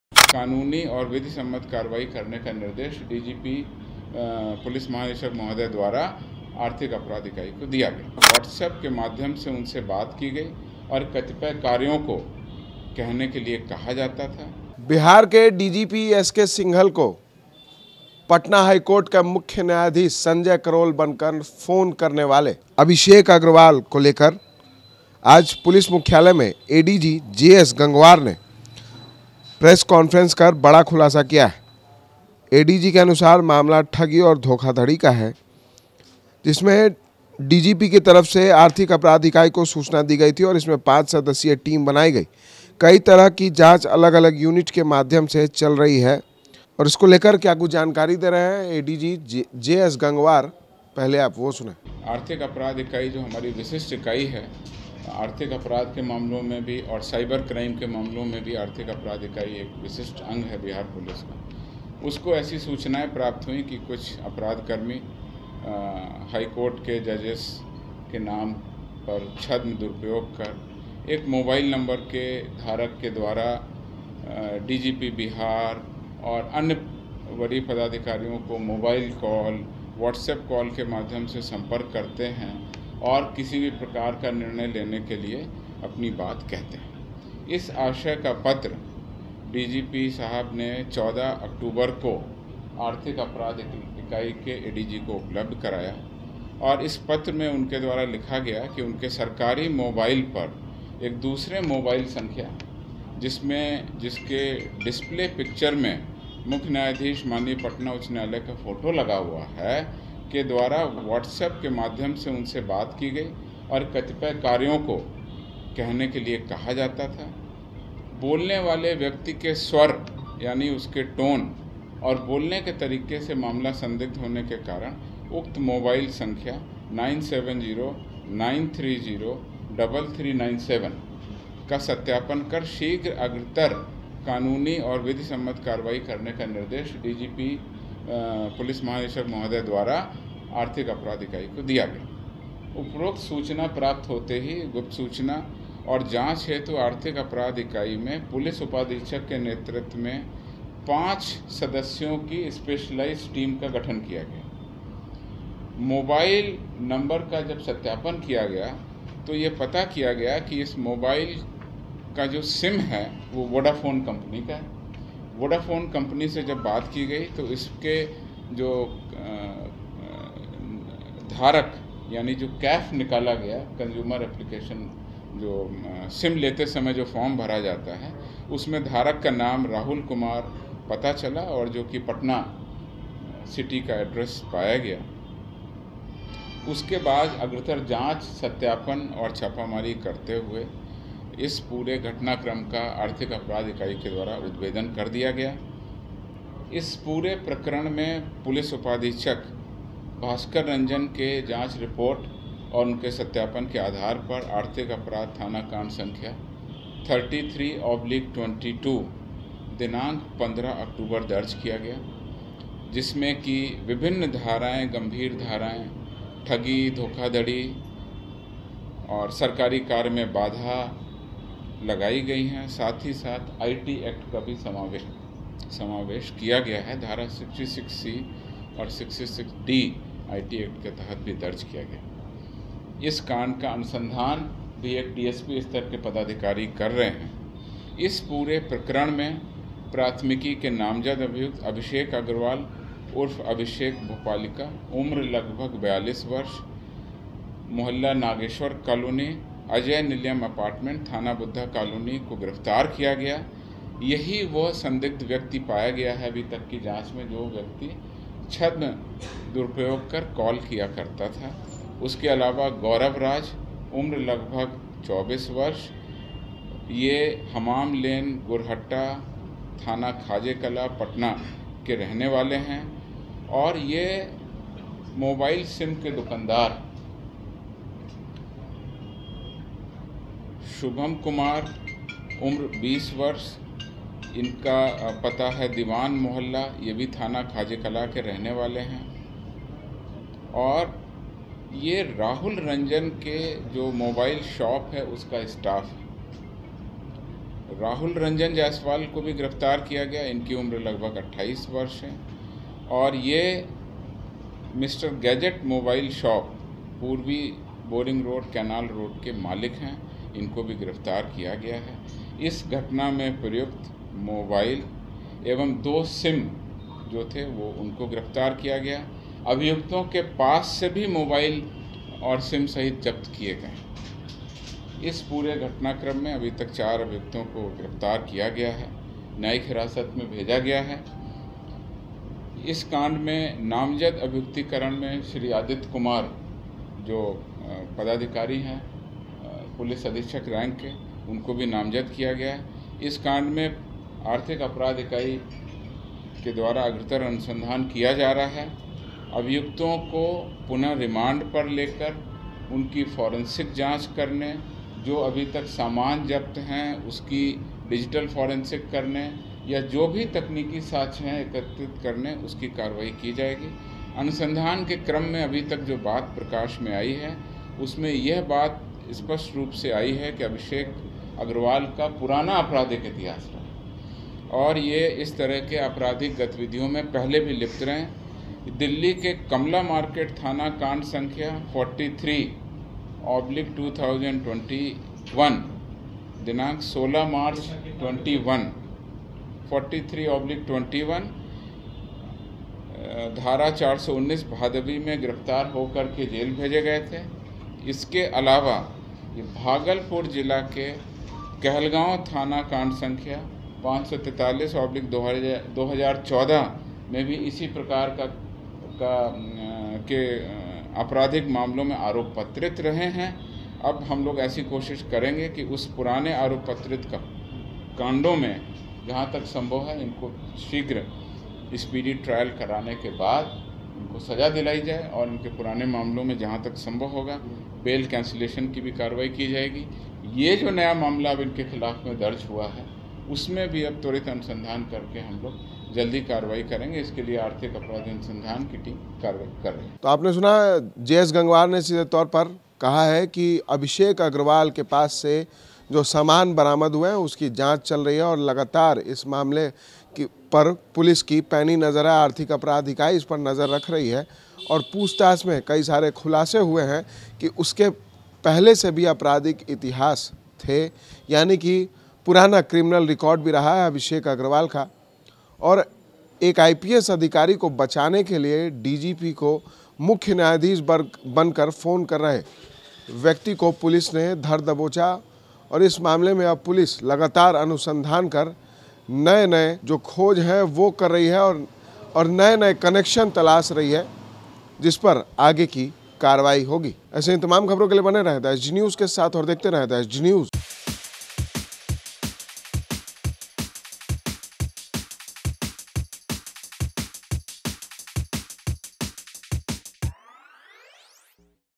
Patna DGP SK Singhal को कहता था "तुम ये करो.."ADG GS Gangwar ने पूरा मामला ही क्लियर कर दिया,देखिये
Edited By: Akash kumar,Himanshu kumar
Anchor: